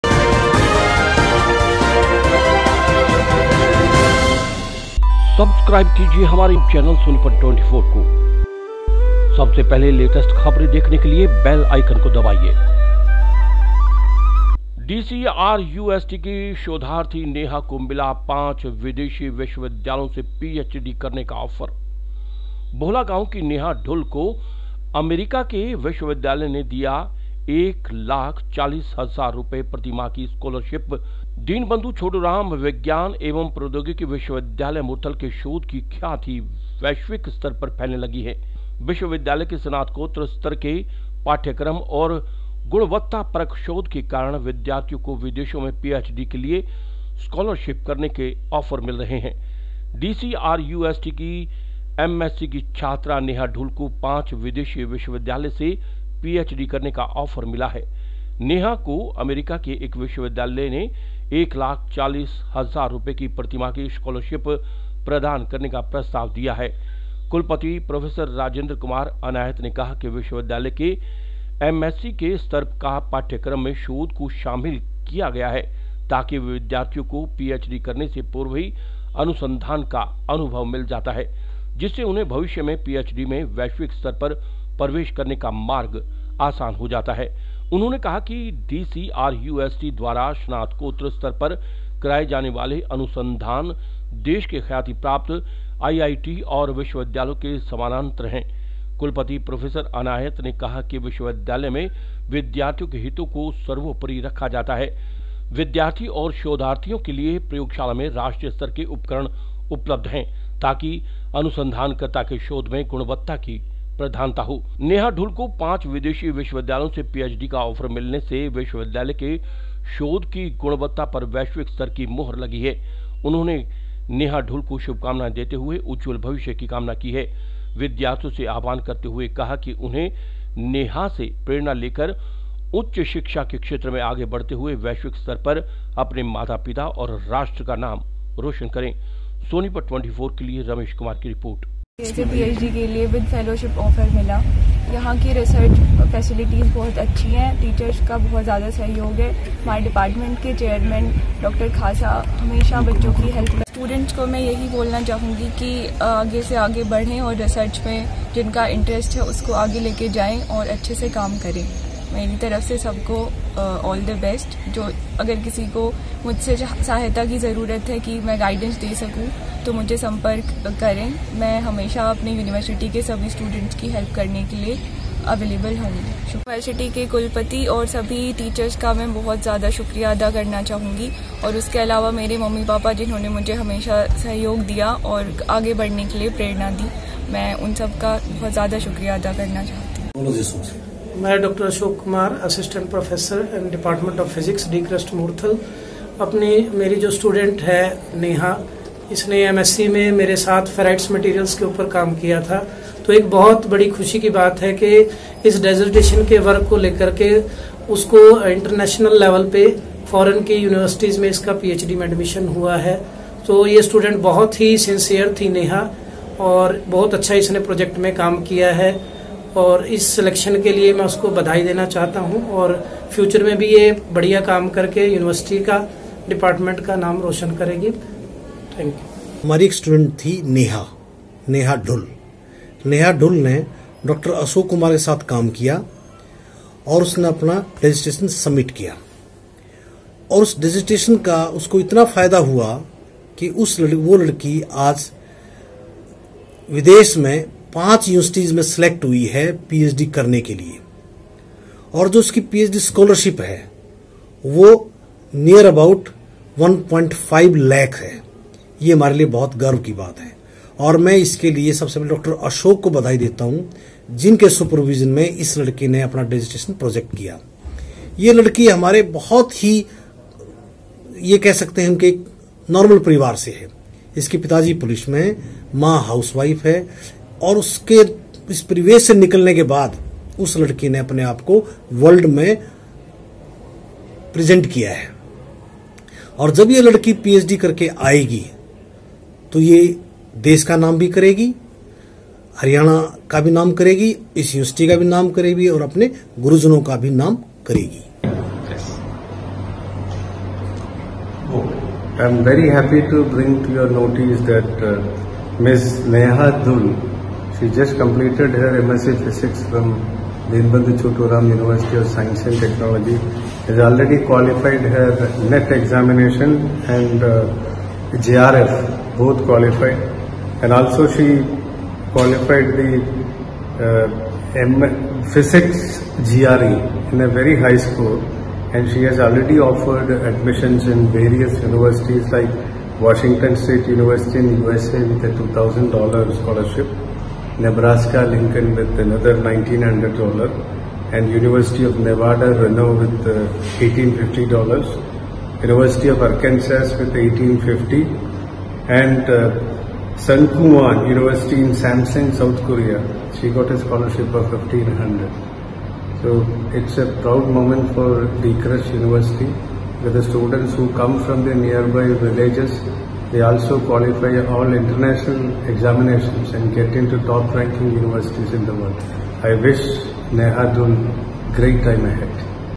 बेटियाँ कर रही नाम रोशन ||sonipat24
बोहला गांव की नेहा ढूल को अमेरिका के विश्वविद्यालय ने दिया 1.40 लाख रुपए प्रतिमाह की स्कॉलरशिप प्रस्ताव

दीनबंधु छोटू राम विज्ञान एवं प्रौद्योगिकी विश्वविद्यालय,मुरथल के शोध की ख्याति वैश्विक स्तर पर फैलने लगी है। नेहा को अमेरिका के एक विश्वविद्यालय ने 1.4 लाख रुपए की प्रतिमाह की स्कॉलरशिप प्रदान करने का प्रस्ताव दिया है।
कुलपति प्रो.राजेंद्रकुमार अनायत ने कहा कि विश्वविद्यालय के एमएससी के स्तर का पाठ्यक्रम में शोध को शामिल किया गया है, ताकि विद्यार्थियों को पीएचडी करने से पूर्व ही अनुसंधान का अनुभव मिल जाता है,जिससे उन्हें भविष्य में पीएचडी में वैश्विक स्तर पर प्रवेश करने का मार्ग आसान हो जाता है। उन्होंने कहा कि डीसीआरयूएसटी द्वारा स्नातकोत्तर स्तर पर कराए जाने वाले अनुसंधान देश के ख्याति प्राप्त आईआईटी व विश्वविद्यालयों के समानानंतर हैं।
कुलपति प्रो.अनायत ने कहा कि विश्वविद्यालय में विद्यार्थियों के हितों को सर्वोपरि रखा जाता है। विद्यार्थी और शोधार्थियों के लिए प्रयोगशाला में राष्ट्रीय स्तर के उपकरण उपलब्ध हैं, ताकि अनुसंधानकर्ता के शोध में गुणवत्ता की प्रधानता हो। उन्होंने नेहा ढूल को शुभकामनाएं देते हुए उज्ज्वल भविष्य की कामना की है। विद्यार्थियों से आह्वान करते हुए कहा कि उन्हें नेहा से प्रेरणा लेकर उच्च शिक्षा के क्षेत्र में आगे बढते हुए वैश्विक स्तर पर अपने माता पिता व राष्ट्र का नाम रोशन करें